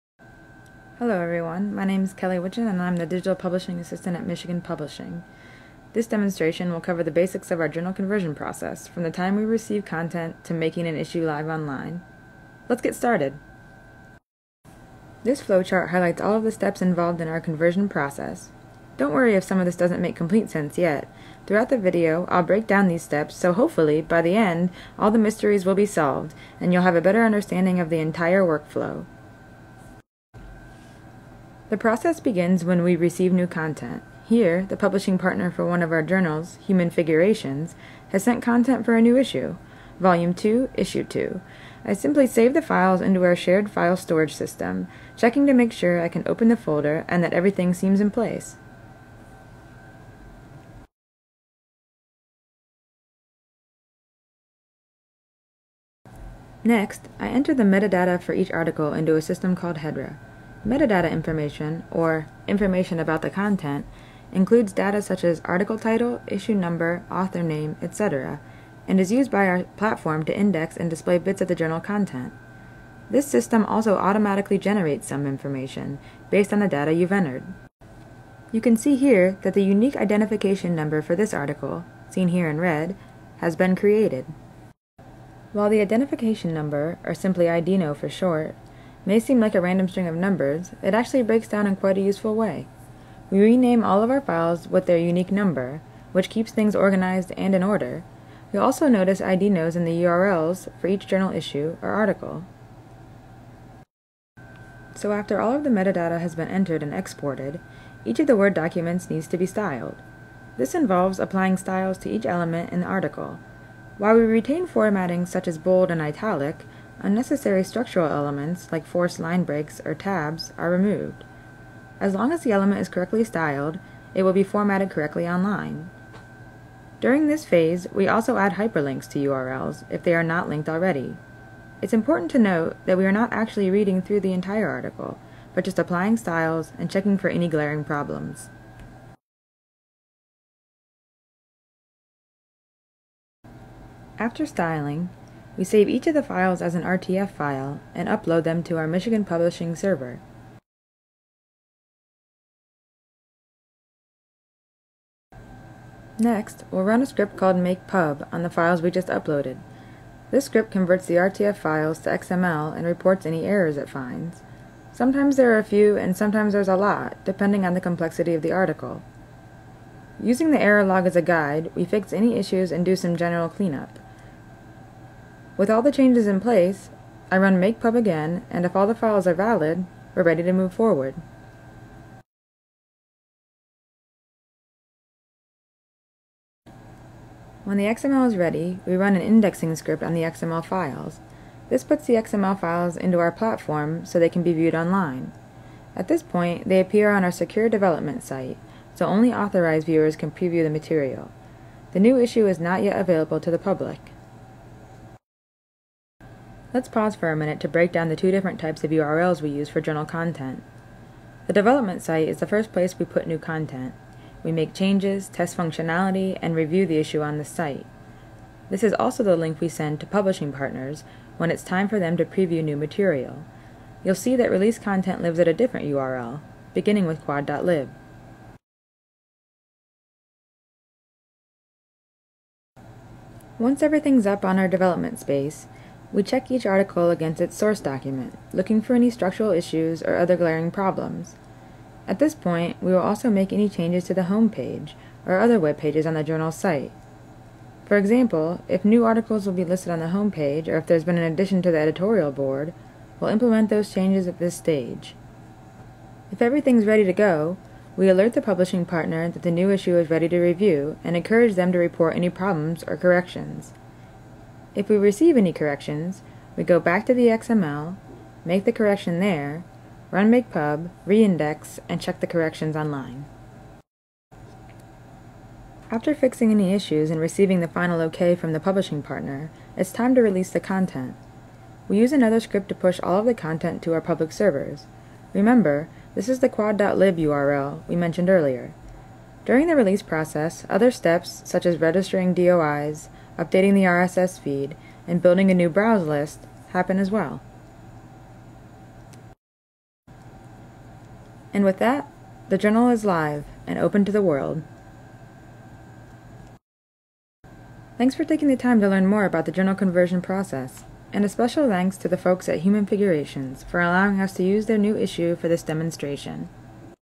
From Word to the Web: Digital Publishing of a Scholarly Journal
This demonstration covers the basics of Michigan Publishing's journal conversion process, from the time we receive content to making an issue live online.

Created by Michigan Publishing